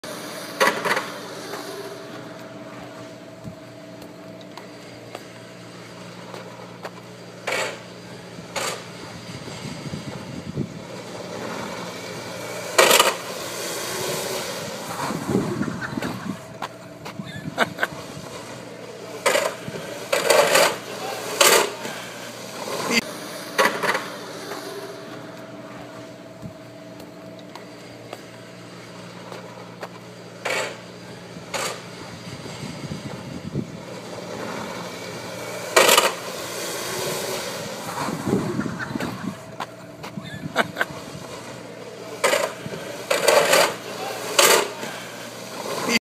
WORLDS QUICKEST SCOOTER?? 😉
Mr Lux with his suped up scooter at Road Atlanta for the Viper Cup!